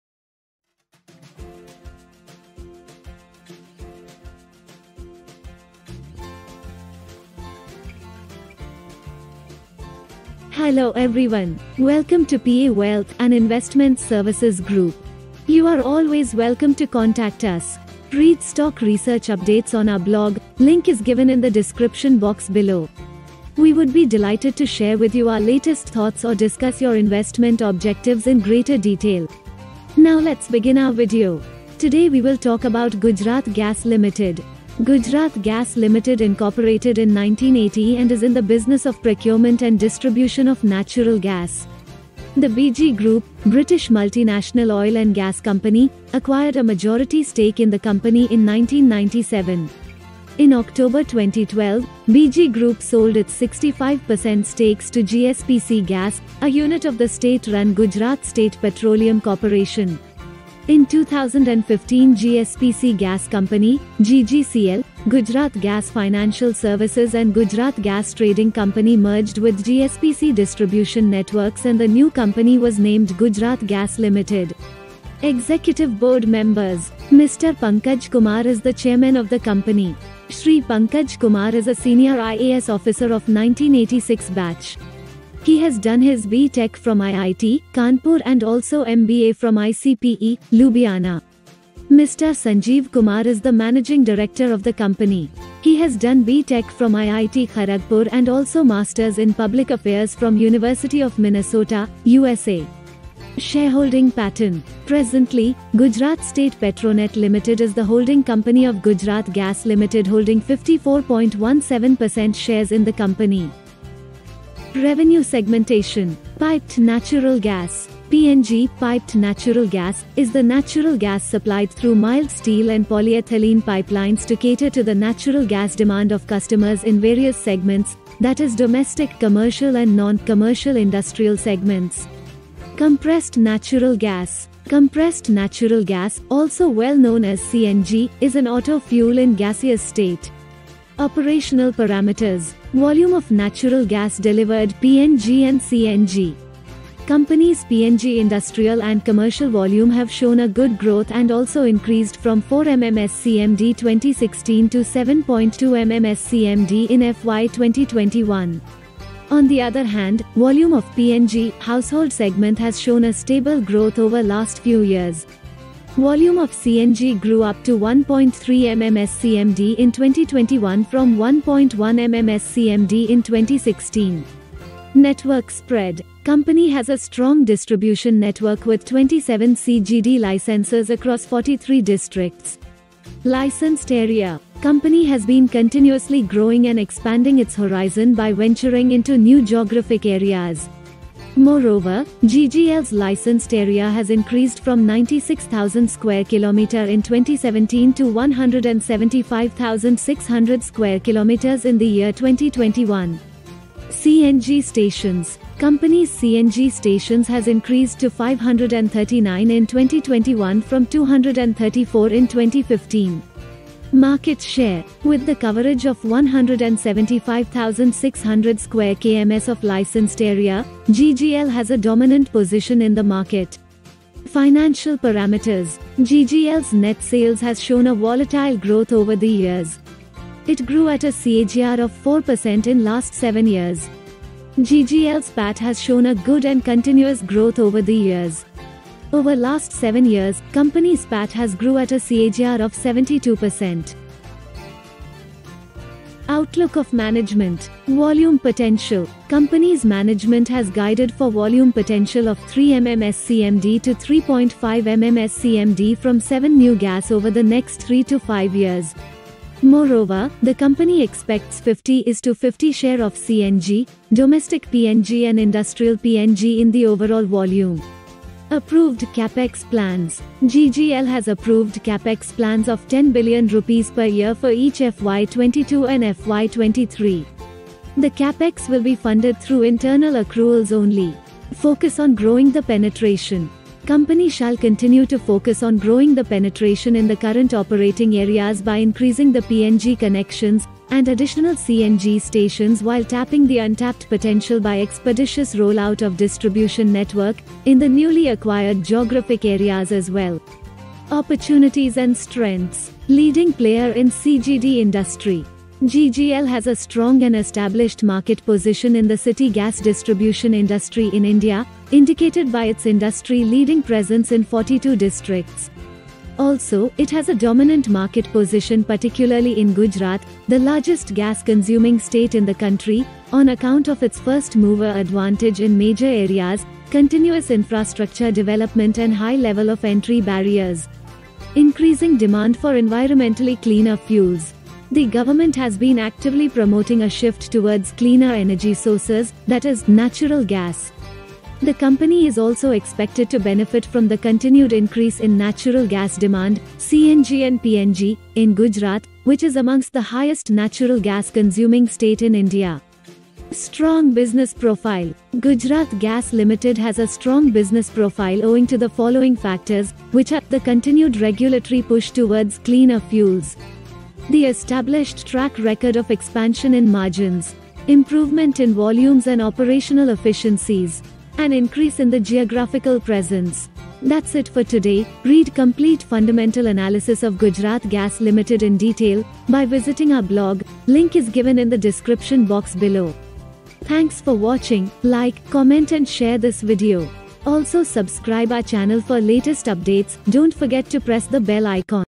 Gujarat Gas Ltd | Fundamental analysis | Stock Market
Read detailed stock research on Gujarat Gas Limited

Contact to know more!

Gujarat Gas Ltd
Stock Analysis
Fundamental analysis
Share News
Owner
Management
History
Gas Company
Overview of operations of Gujarat Gas Ltd and its Business Model
Executive Board Members
Shareholding Pattern
Revenue Segmentation
Operating Parameters of Gujarat Gas Ltd
Cost Structure
Network Spread
Year wise Market Share
Key Financial Highlights
Addition of new geographical areas
Management’s Growth Outlook
Opportunities and Strengths
Risks and Concerns